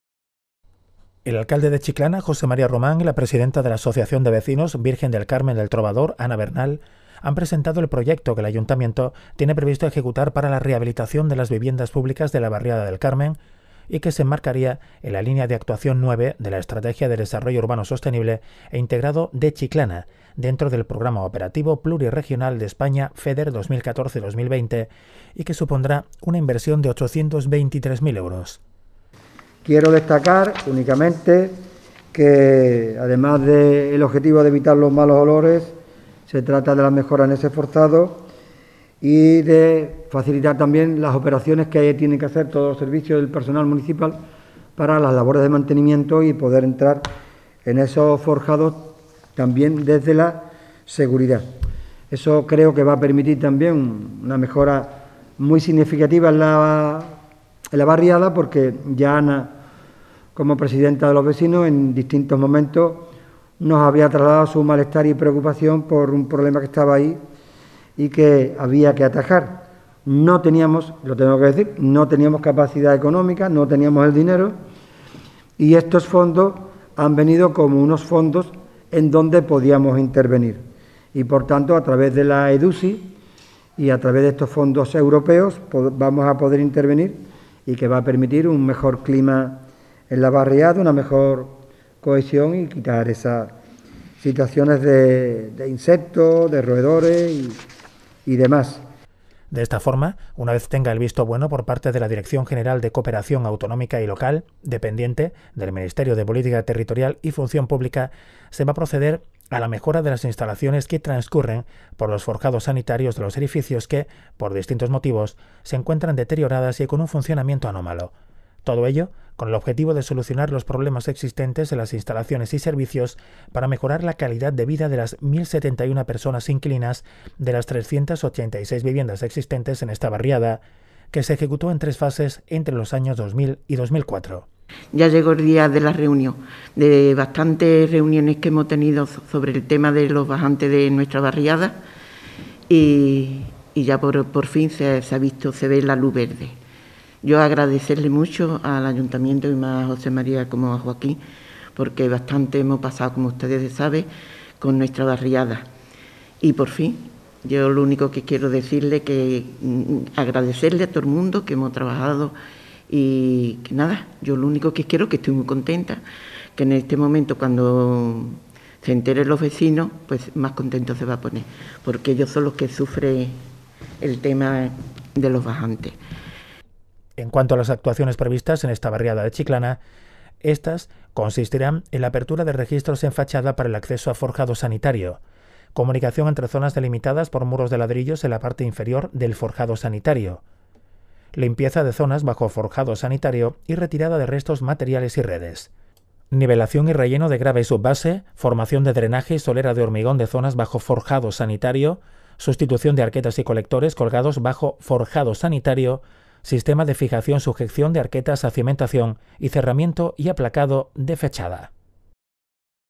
LA BARRIADA VIRGEN DEL CARMEN MEJORARÁ GRACIAS A LA EDUSI
El Ayuntamiento trabaja en un proyecto de rehabilitación y mejora de los forjados sanitarios en la barriada del Carmen. El alcalde y la presidenta de la AVV Virgen del Carmen del Trovador presentan las actuaciones que se prevén ejecutar con cargo a la EDUSI y que solucionarán los problemas de malos olores, insalubridad y plagas.